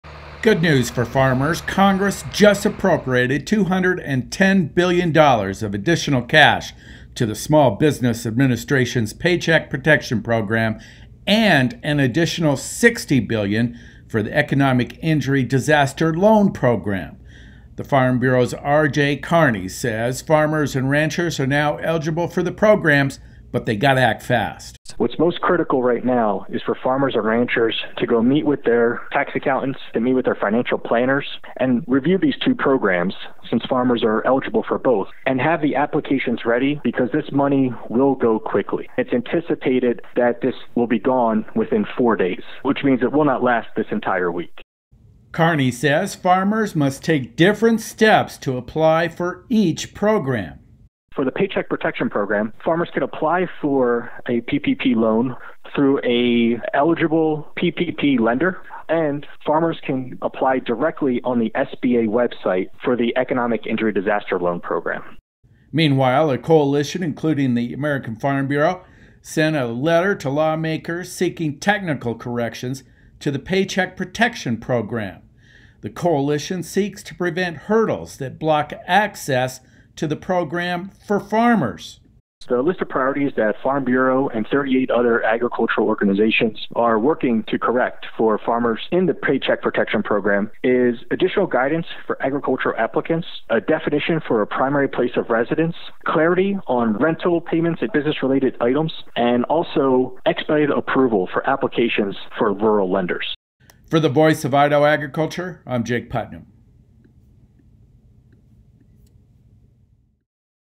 Farmers to apply now to get fed aid
Congress just authorized more federal economic aid for small businesses, including farmers and ranchers.

Congress just appropriated $310 billion of additional cash to the Small Business Administration’s Paycheck Protection Program and an additional $60 billion for the Economic Injury Disaster Loan program. The Farm Bureau’s RJ Karney, says farmers and ranchers are now eligible for the programs but must act fast.

“What’s most critical right now is for farmers and ranchers to go meet with their tax accountants, to meet with their financial planners, and review these two programs, since farmers are eligible for both, and have the applications ready because this money will go quickly. It’s anticipated that this will be gone within four days, which means it will not last this entire week,” said Karney.

 Karney says farmers must take different steps to apply for each program.

“For the Paycheck Protection Program, farmers can apply for a PPP loan through an eligible PPP Lender. And, farmers can apply on the SBA website for the Economic Injury Disaster Loan program,” said Karney

 Meanwhile, a coalition including AFBF Monday sent a letter to lawmakers seeking technical corrections to the Paycheck Protection Program. The coalition seeks to prevent hurdles that block access to the program for farmers.

“The list of priorities that Farm Bureau and 38 other agricultural organizations are working to correct for farmers in the Paycheck Protection Program is additional guidance for agricultural applicants, a definition for a primary place of residence, clarity on rental payments and business-related items, and also expedited approval for applications for rural lenders,” said Karney.